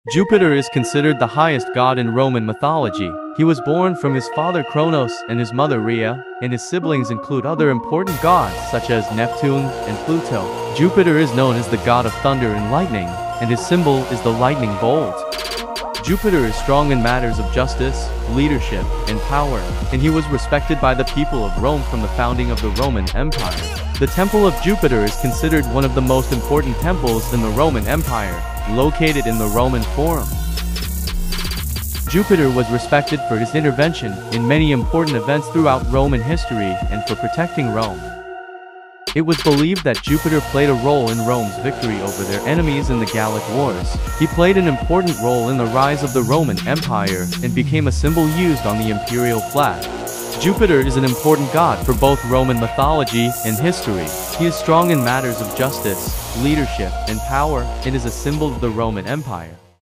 Jupiter: The Protector God of the Roman Empire
This video provides information about Jupiter, one of the most important gods in Roman mythology. Jupiter is considered the protector god of the Roman Empire and played a significant role in many important events throughout Roman history. The video offers detailed information about Jupiter's powers, symbols, temple, and role in Roman history. Additionally, it provides resources for those who want to learn more about Roman mythology and history. It is designed for those who want to watch an informative and engaging video.